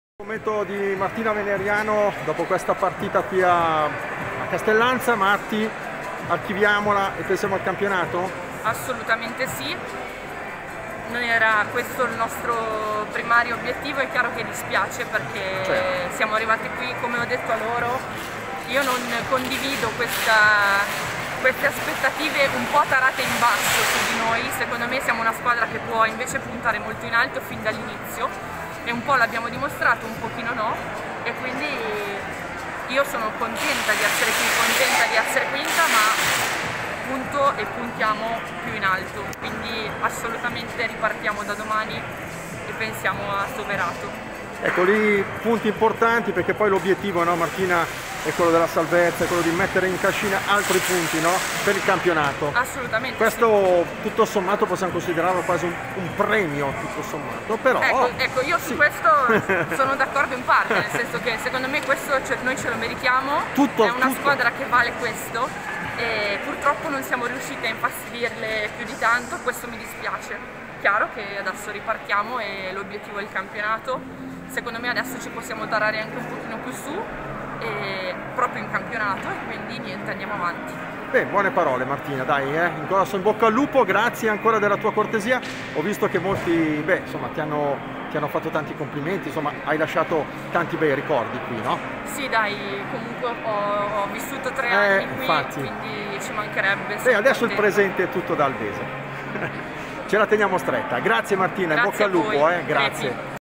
LO SPIRITO DA "GUERRIERA" DI MARTINA:"PUNTIAMO IN ALTO, ABBIAMO GRANDI QUALITA': RIPARTIAMO COSI'"
Il commento di Veneriamo dopo la sconfitta in Coppa Italia a Castellanza:"Adesso pensiamo a Soverato ed a fare bene sabato"